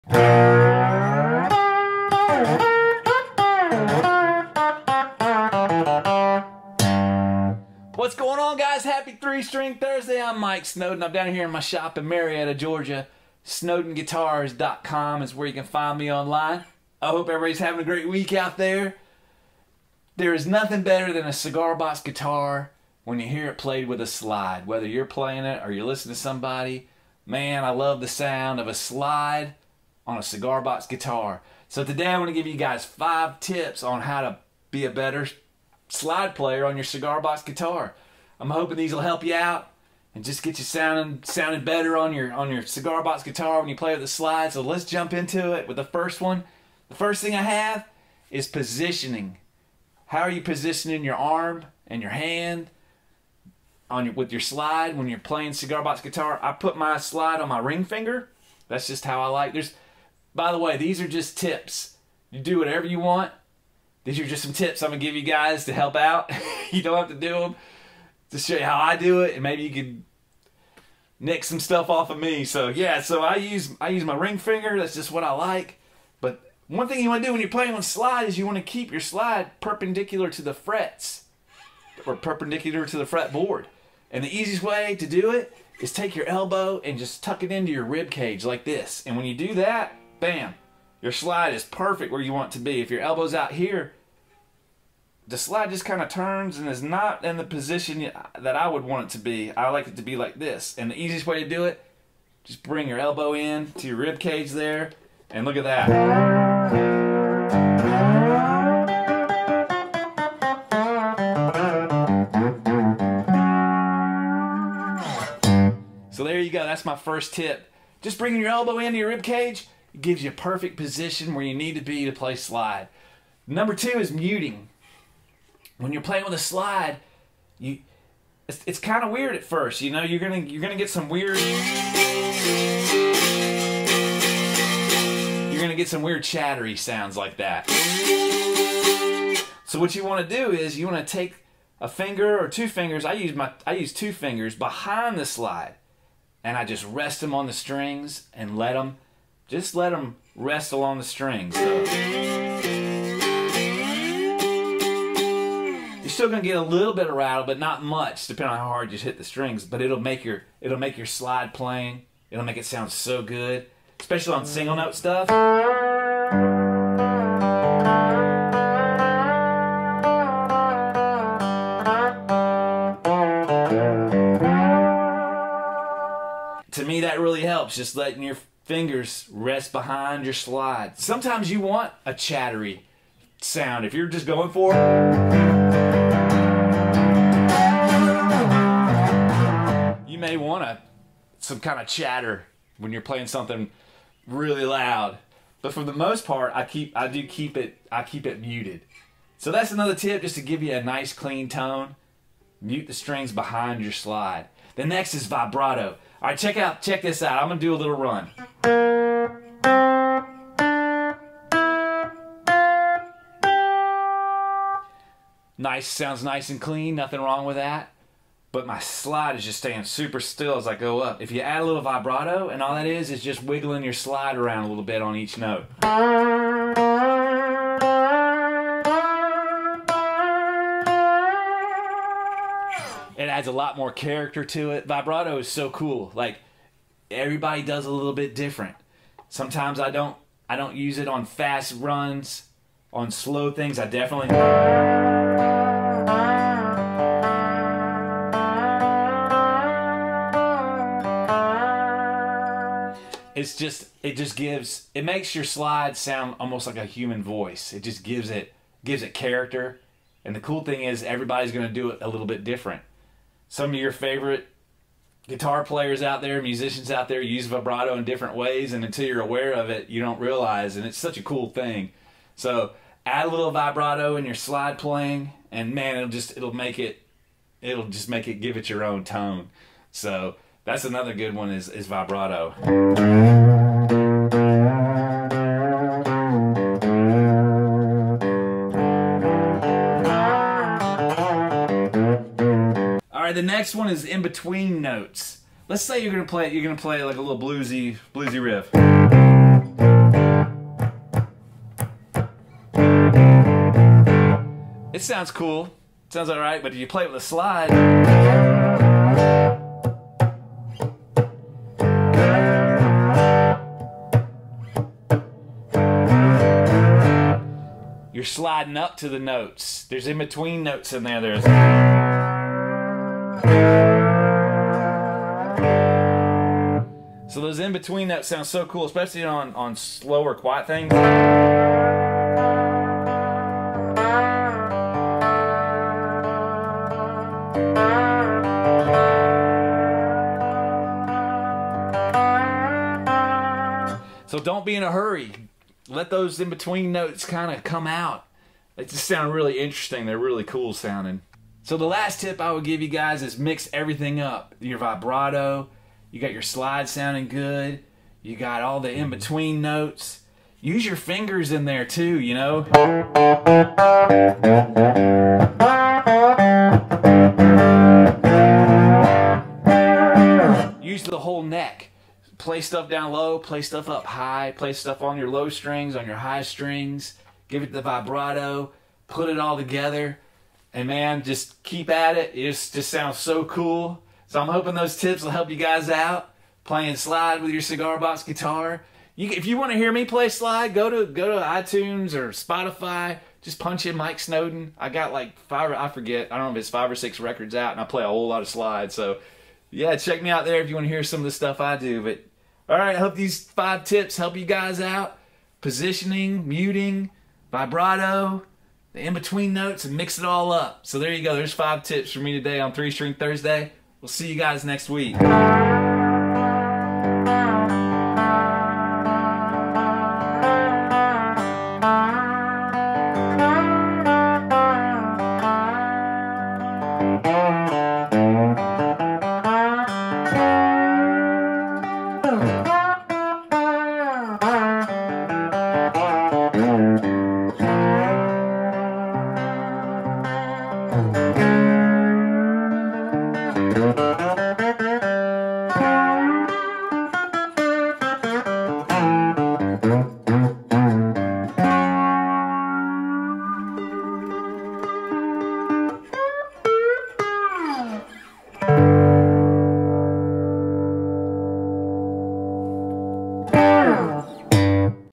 5 Tips for Playing Slide on a Cigar Box Guitar with Mike Snowden Open G Tuning G-D-G
5 Tips for Playing Slide on a Cigar Box Guitar with Mike Snowden

1. Positioning
2. Muting Strings
3. Vibrato
4. In-between Notes
5. Mix It UP!

Happy 3 String Thursday with Mike Snowden - Snowden Guitars.

Check out my cigar box guitar music on iTunes and Spotify!